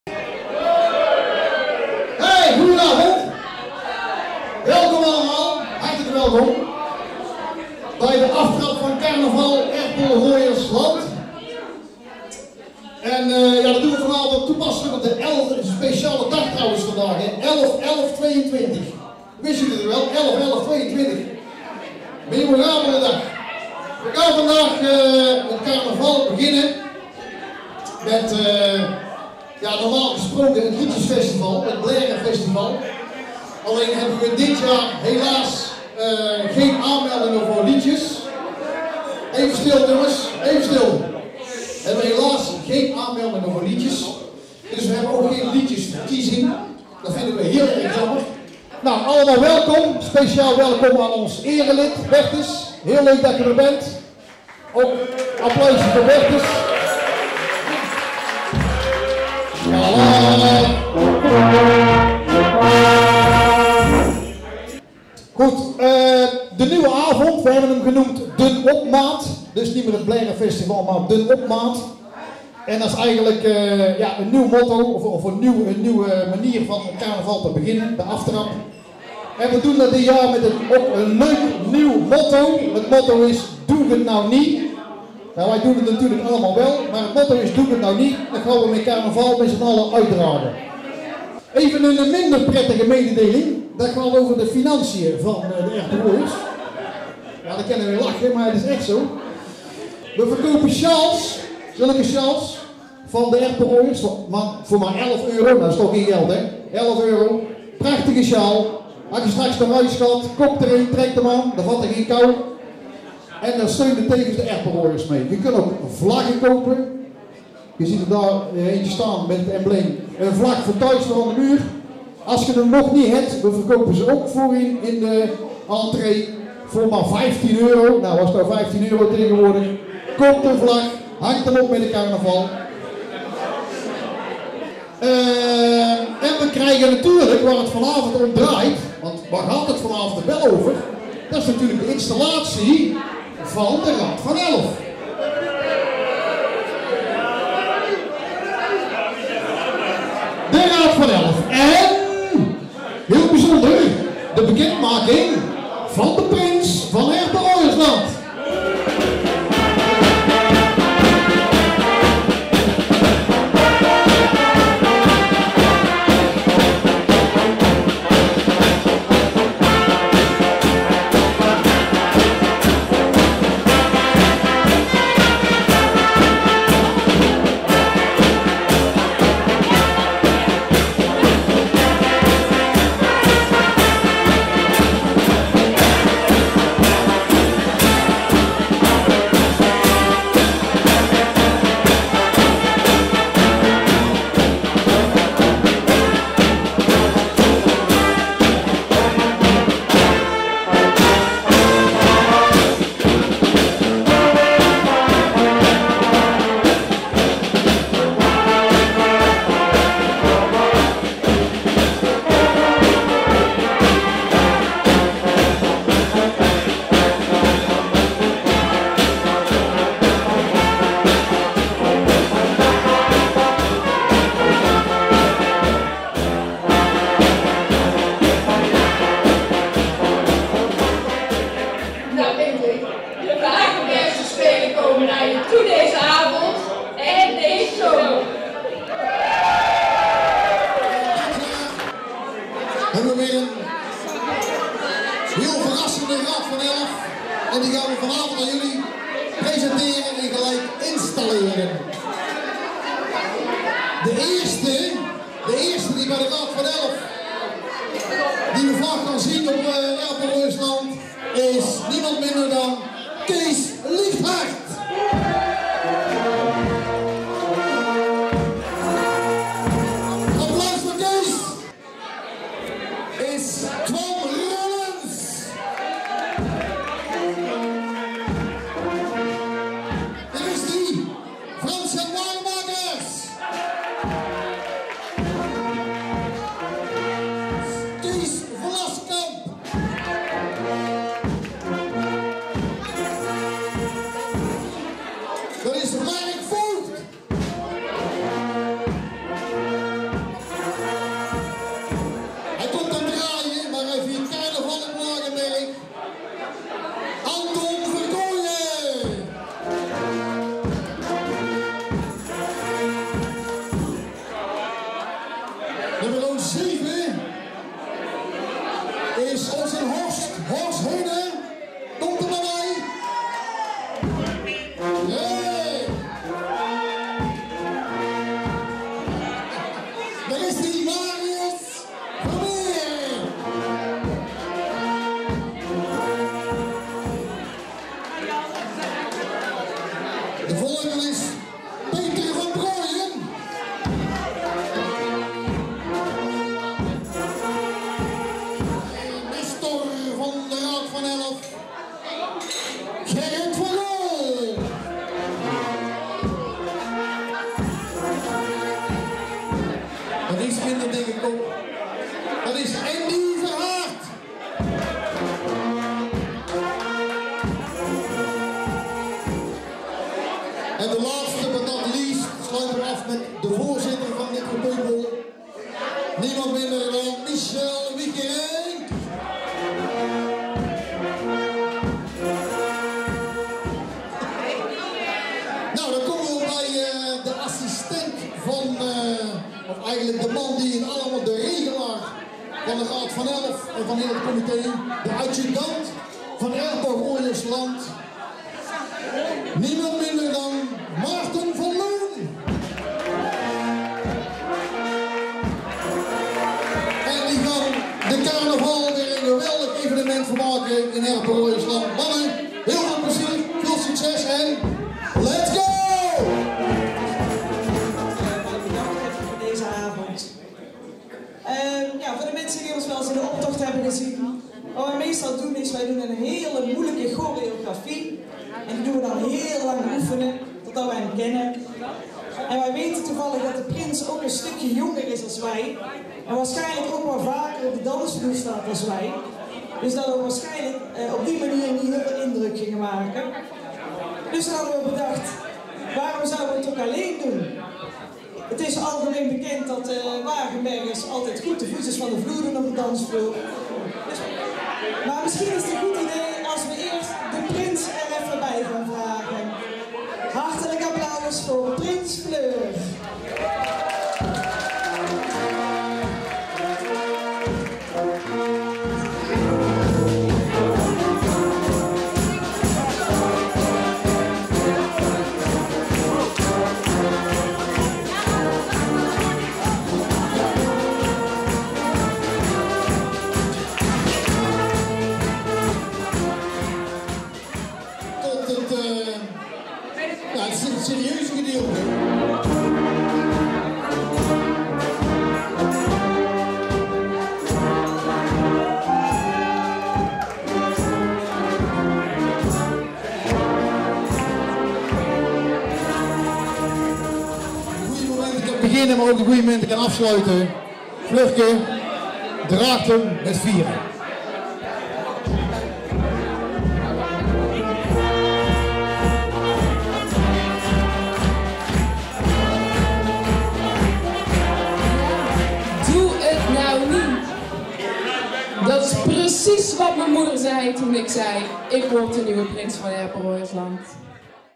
11 -11 Wagenberg
11e van de 11e bal Terheijden, Made, Lage Zwaluwe en Wagenberg
De Schraansers in Terheijden hebben carnaval 2023 ingeluid. Op 11 november was het groot feest, met onder meer de onthulling van de jeugdraad; Festivalaaf; en natuurlijk de  onthulling van de Prins door cv de Braoipotte. De stemming zat er goed in en het belooft dan ook weer een carnavalesk feest te worden in 2023.
Ook in Wagenberg, Lage Zwaluwe en op De May werd het Carnaval spetterend ingeluid. Wij waren erbij en feestten mee. Alaaf!